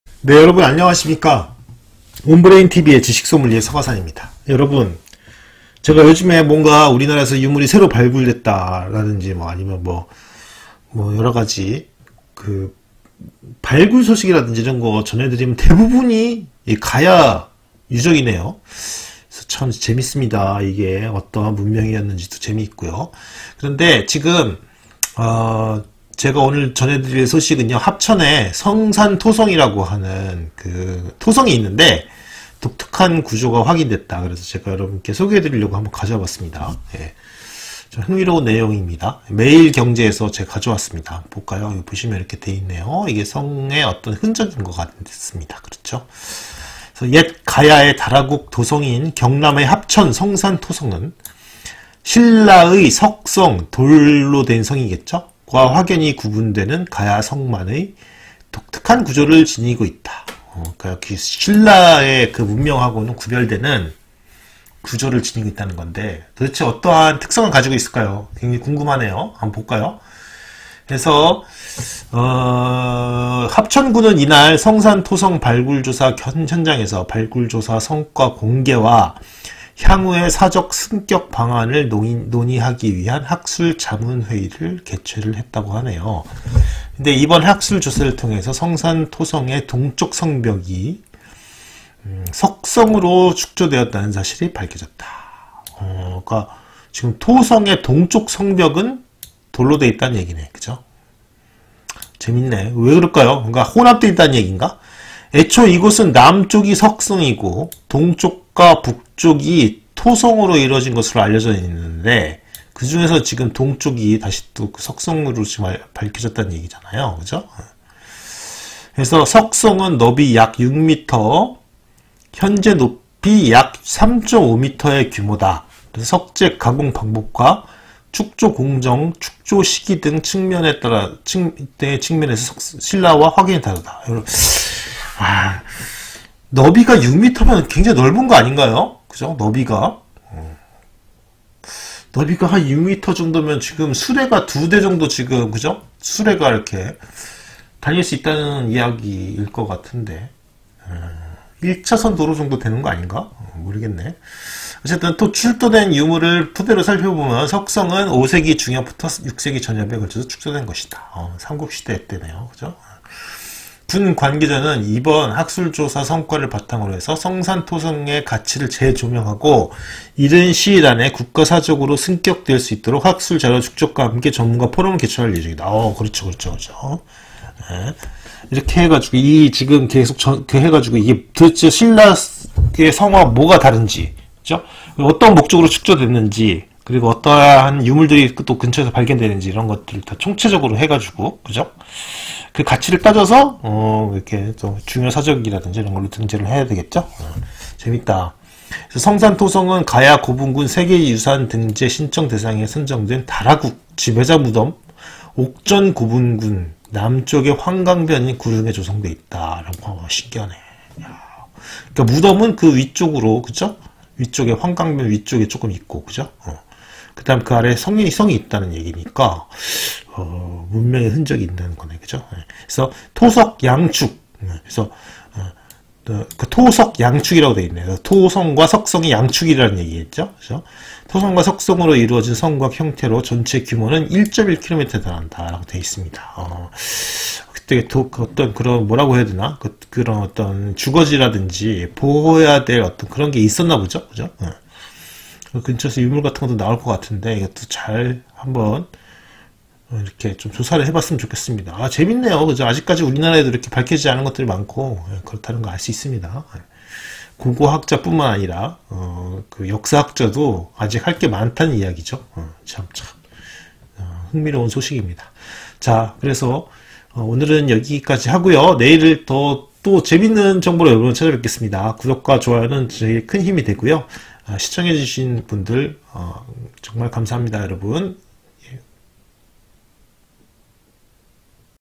합천의 성산토성은 참 특이하다고 합니다.
가야의 성은 신라의 성과 다르다는데, 어떻게 다른지 살펴봅시다^^